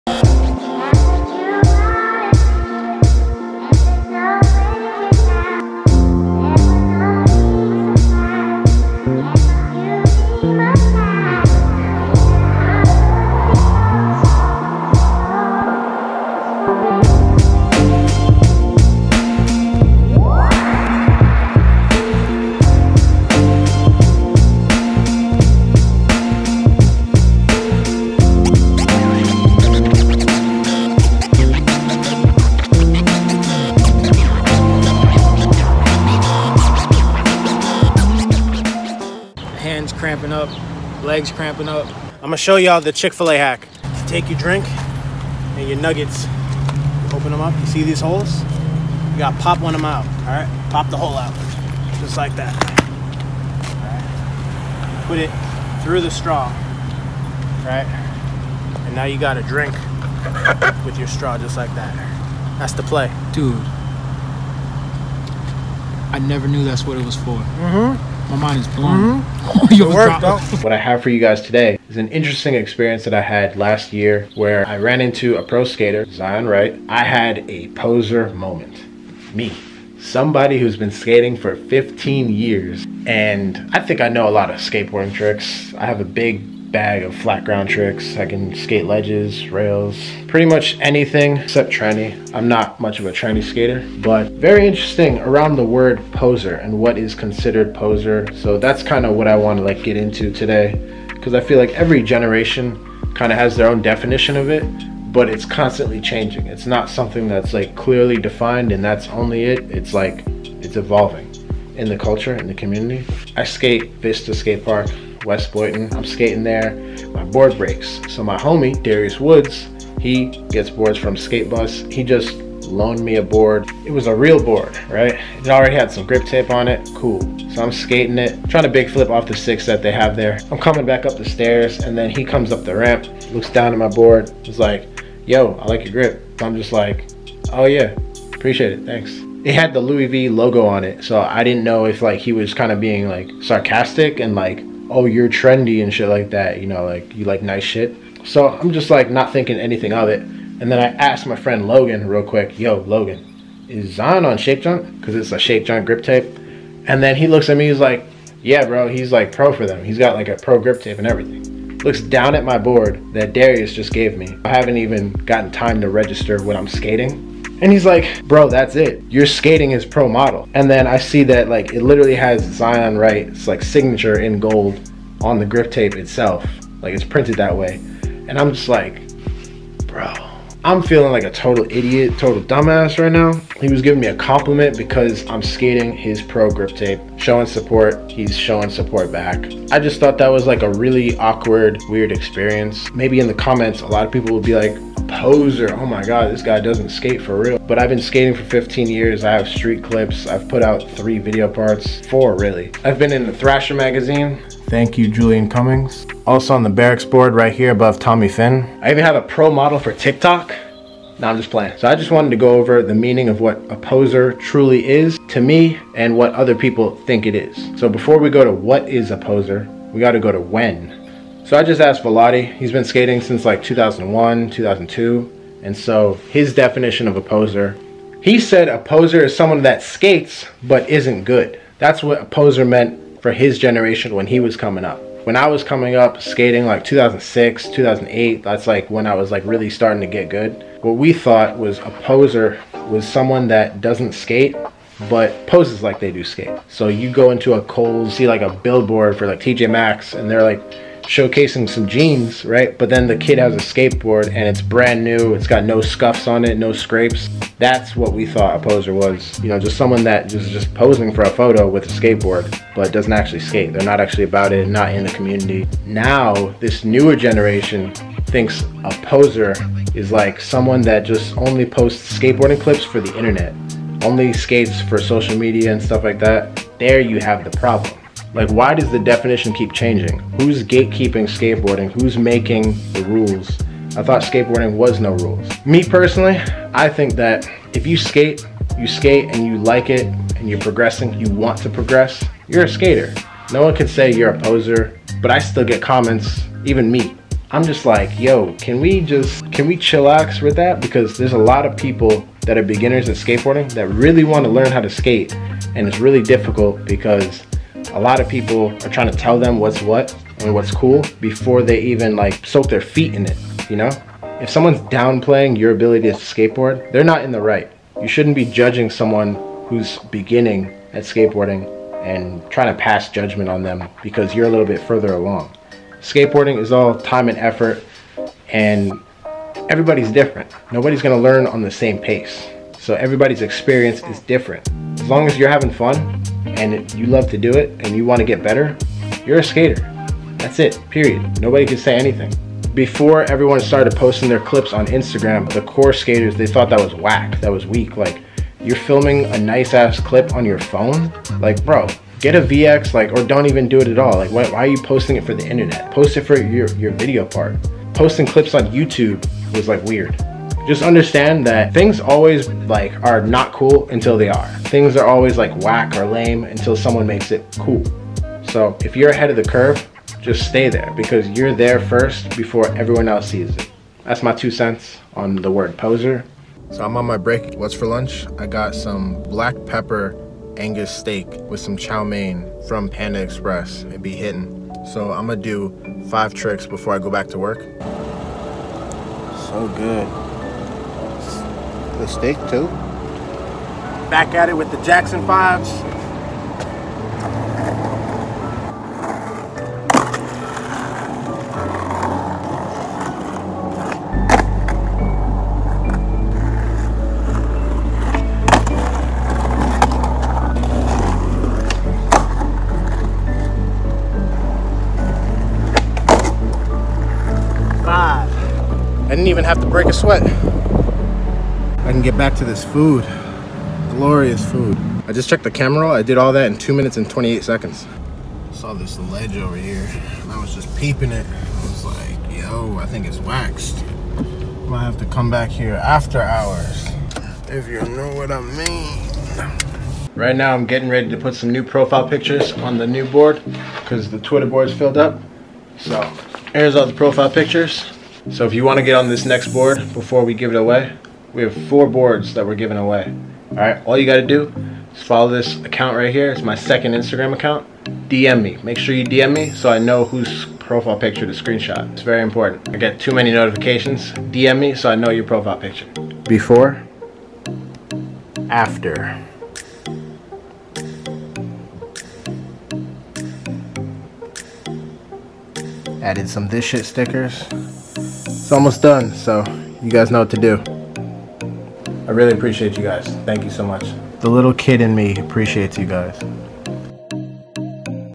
My Poser Story with RedBull Athlete Zion Wright
Had an amazing Go Skate Day Jam sesh. In this video I tell an awkward poser moment that was far too recent for a skate veteran like me to be okay with but thought I would share my story to make a point. Why does the meaning of a poser change each generation? It's okay to be beginner skater, & you don't have to call them a posers for trying. Remember it is only after you eat sht skateboarding that you got you your tough skin. Remember your humble beginnings on a skateboard & don't be a dickhead.
Please thumbs up this video  !!